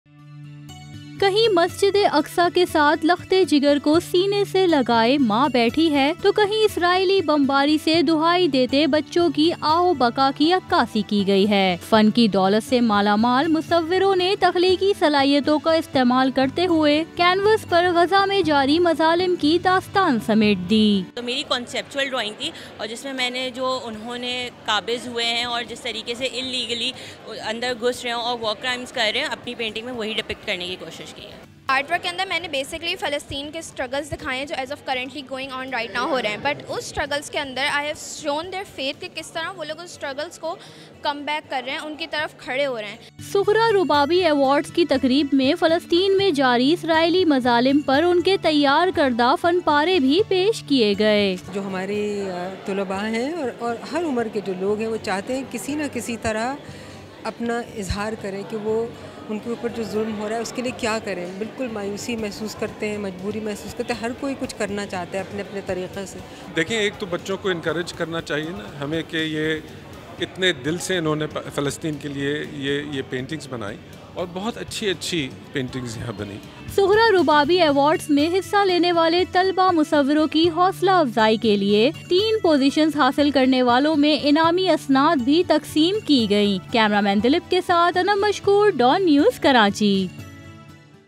Young Artists Paint For Palestine In Exhibition | Sughra Rababi Award Ceremony | Dawn News
Palestine Per Muzalim Ki Tarjumani Kartay Talba Kay Fun Paray | Dawn News

فلسطینیوں پر قیامت خیز مظالم کی ترجمانی کرتے فن پارے
طلبا کے بنائے فن پارے توجہ کا مرکز